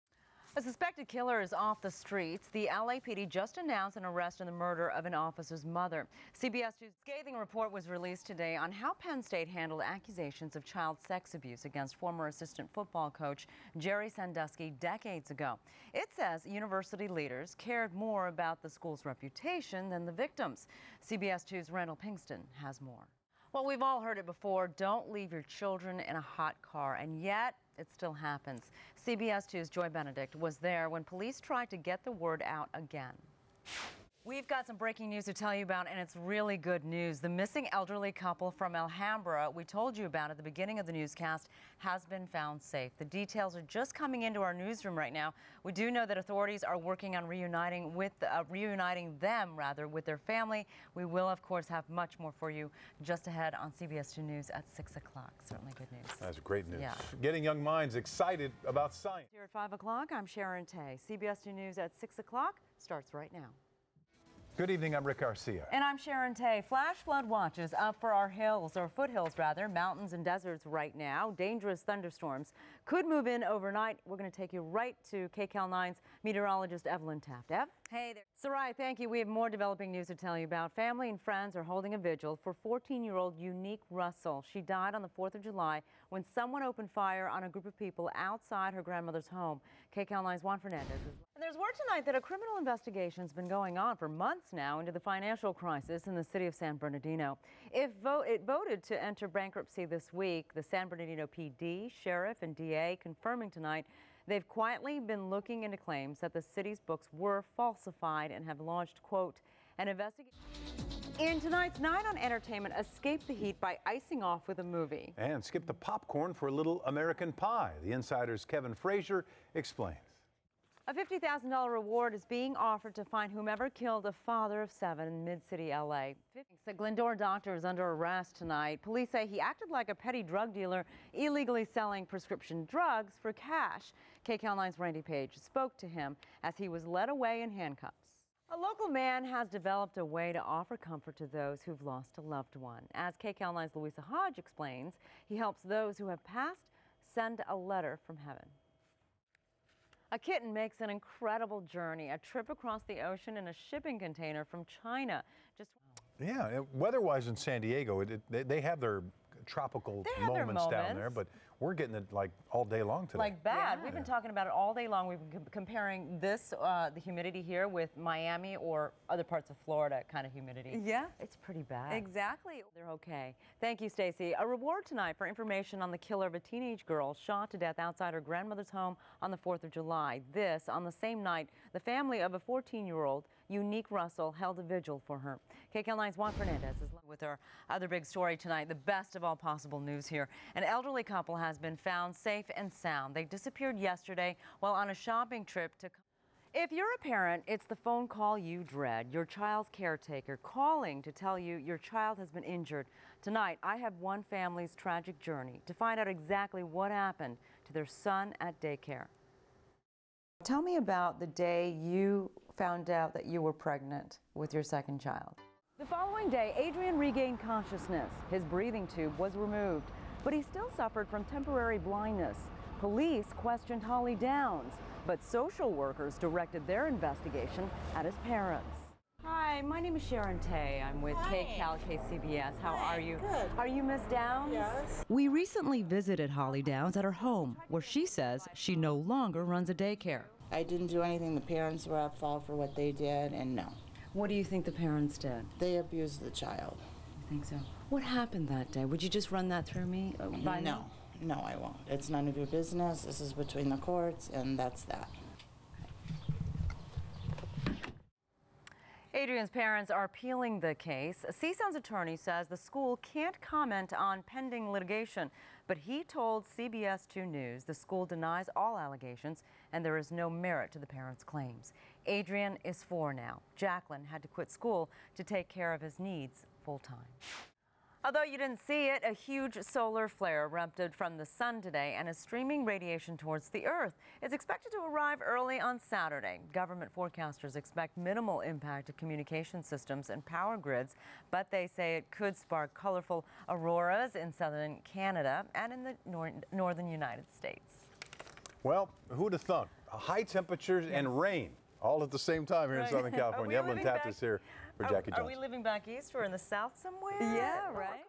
Sharon Tay 2012/07/12 KCAL9 HD
Sharon Tay, Los Angeles TV news anchor on KCAL/CBS. Air date: July 12, 2012.